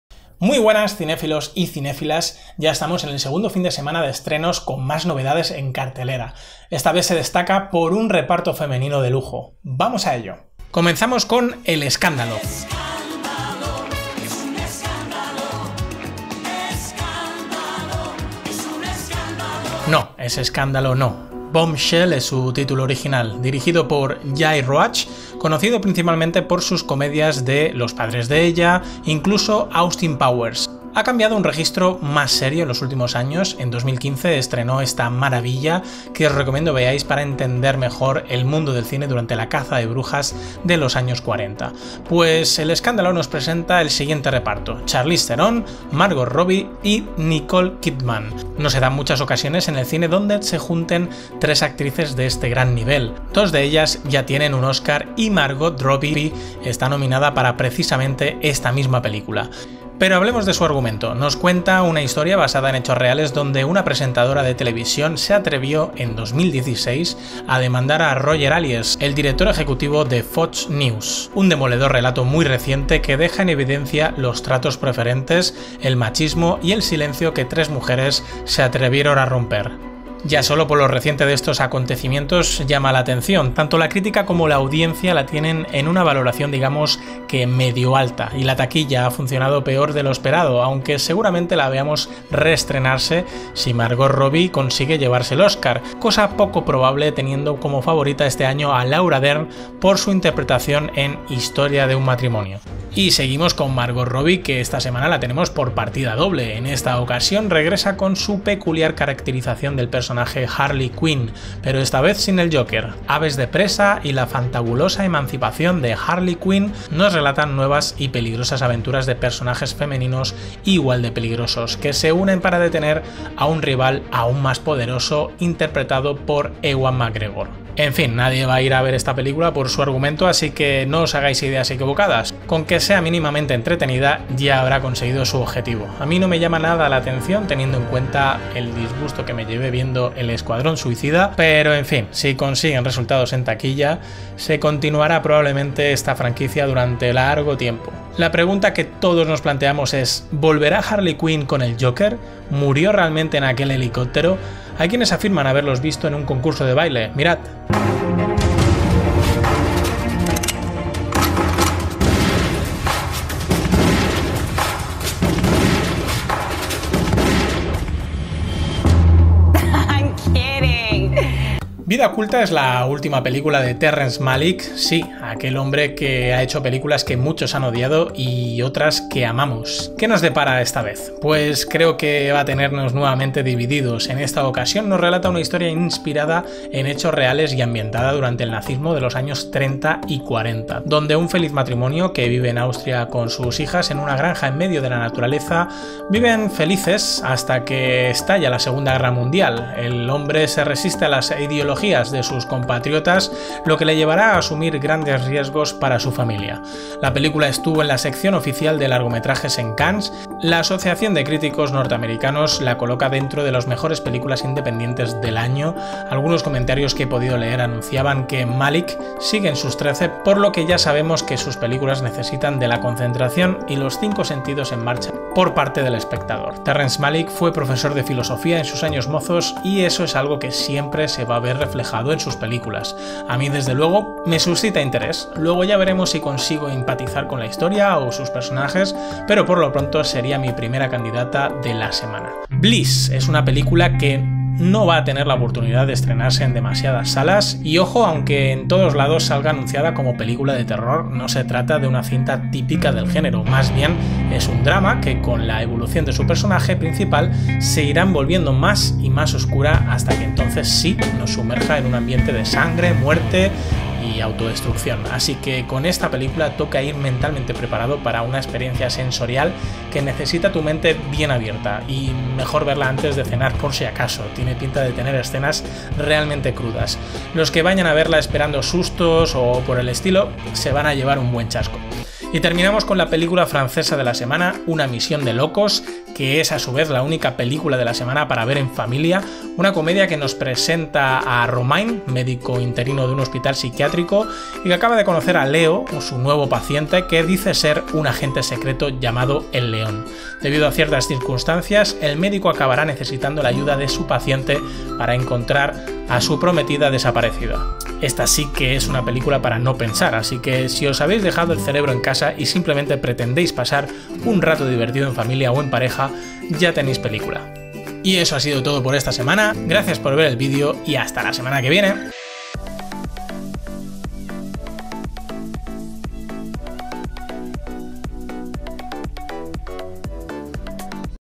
Estrenos de cine 20200207
Cartelera de la semana. Los estrenos en España (y probablemente más países) más importantes.